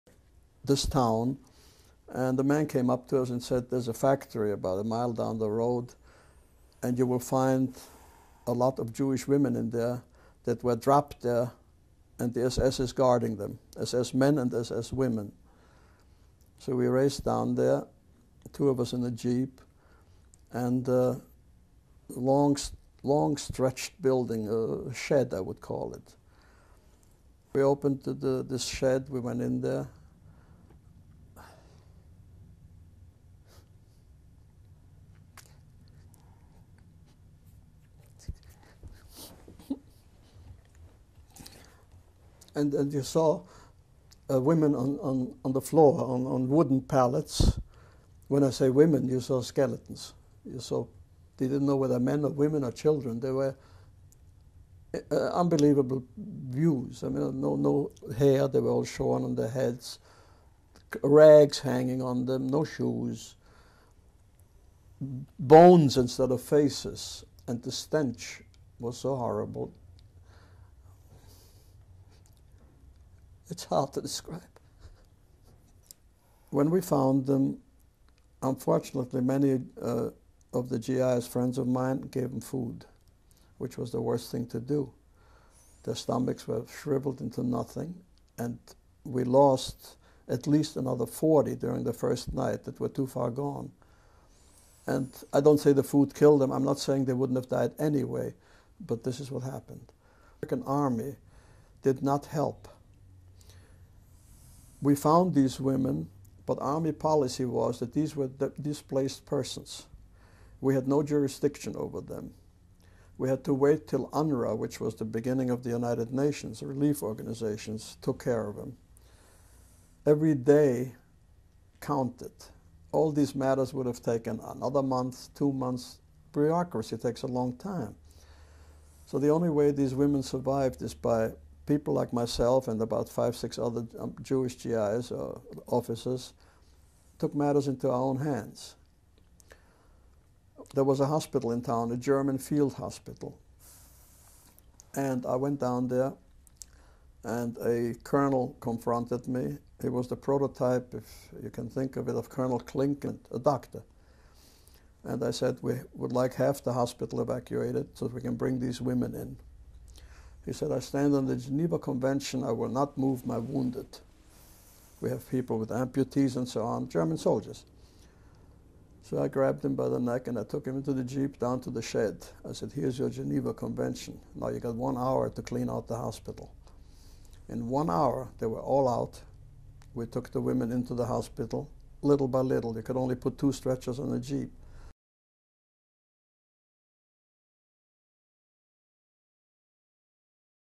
US Army veteran Harry Mogan recalls about his first encounter with Nazi atrocities
On 20 January 1945, approximately 1000 female Jewish prisoners were evacuated from the Schlesiersee (today Sława) camp in Upper Silesia, western Poland, a region annexed to Germany. These women were forced on a death march in a southwesterly direction. On the way, the prisoners passed through other camps, and more women were added to the march.
On 5 May 1945, after covering a distance of over 800 km, the march ended in the town of Volary (German: Wallern) in Czechoslovakia, not far from the border with Germany and Austria.
Of the approximately 1,300 women who marched to Volary, some 350 survived. .
Video Credit: USC Shoah Foundation – The Institute for Visual History and Education